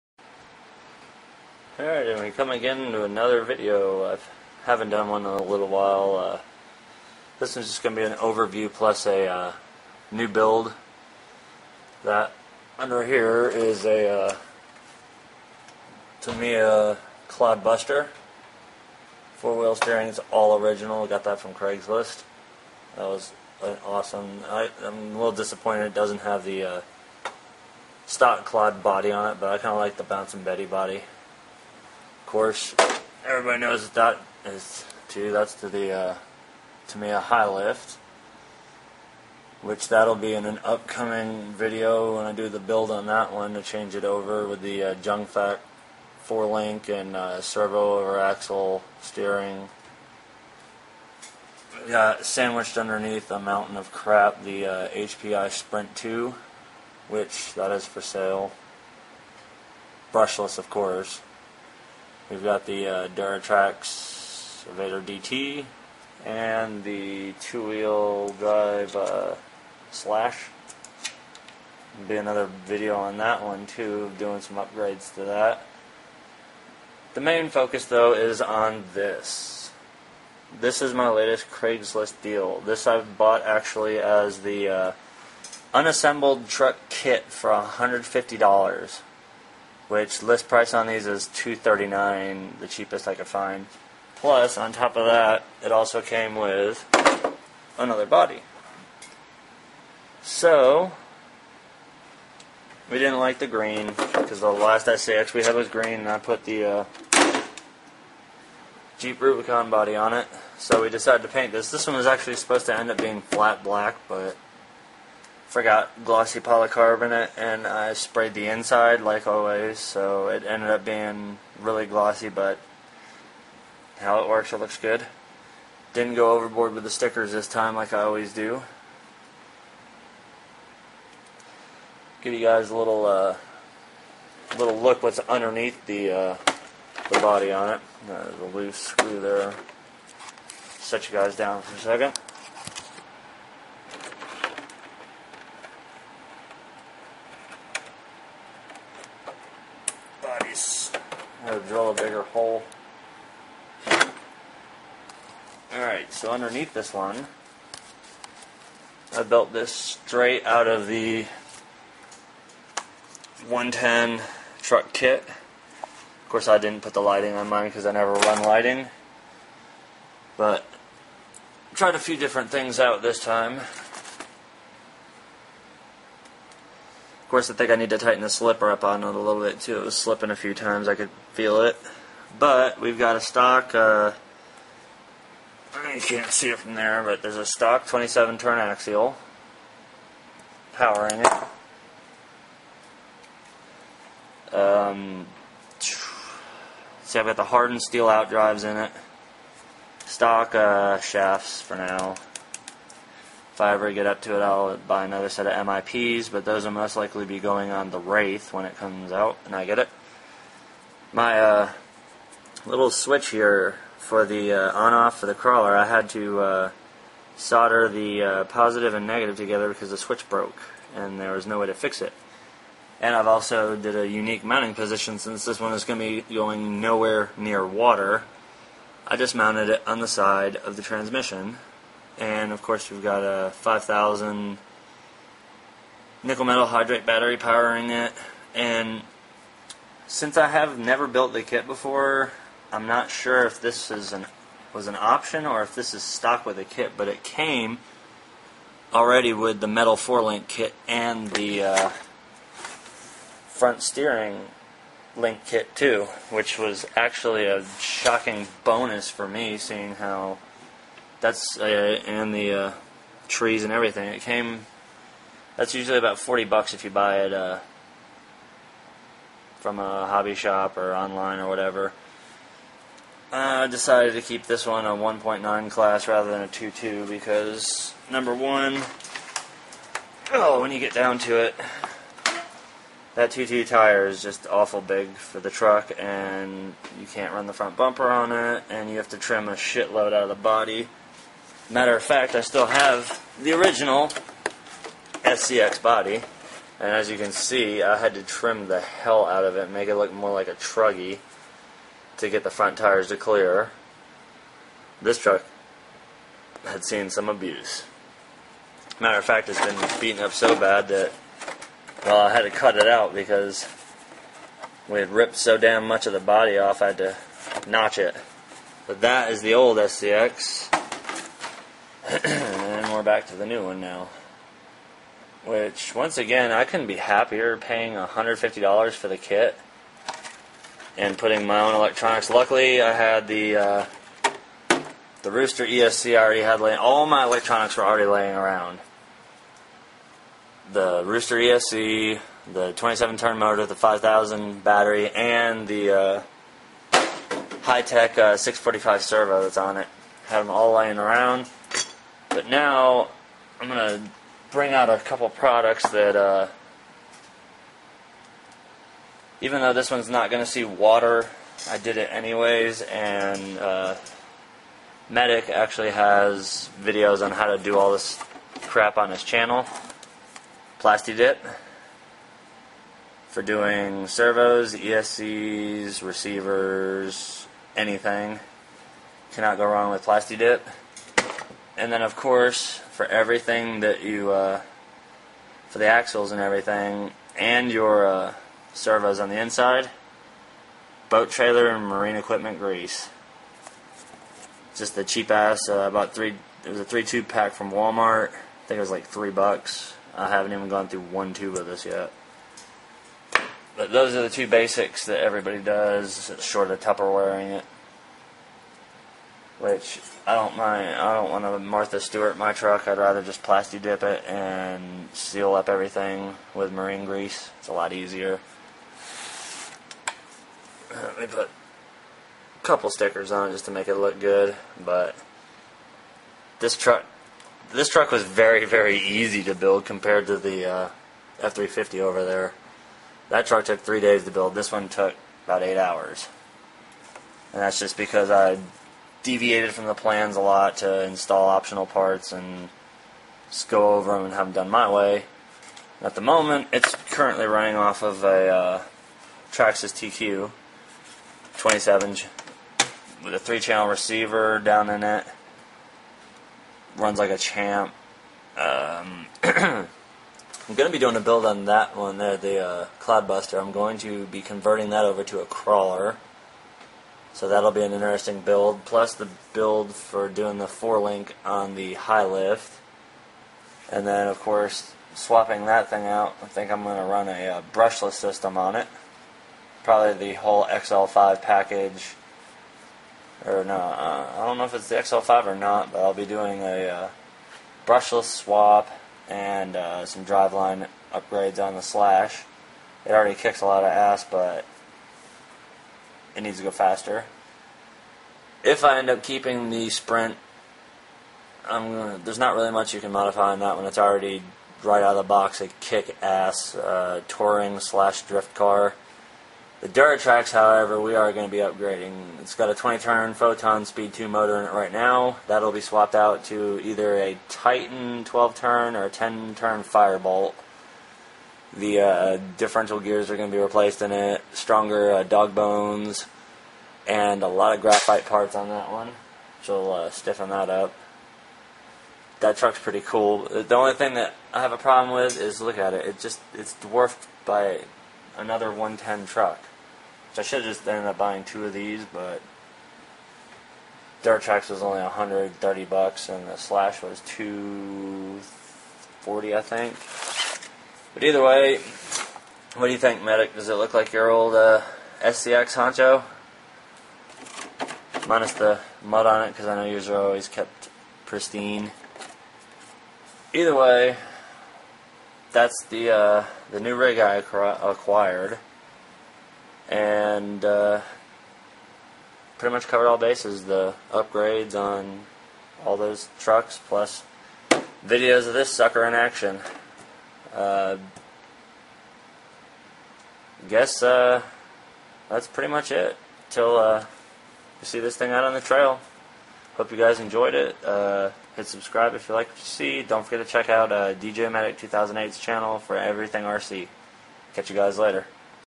Craigslist SCX10 build
I got this truck off of craigslist for 150 bucks, had spare electronics lying around and got to building.